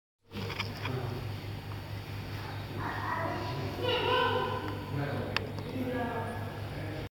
Zoo Barcelona - Demos Casio FH20
Demostraciones en el Zoo de Barcelona de la nueva cÃ¡mara Casio Exilim FH20.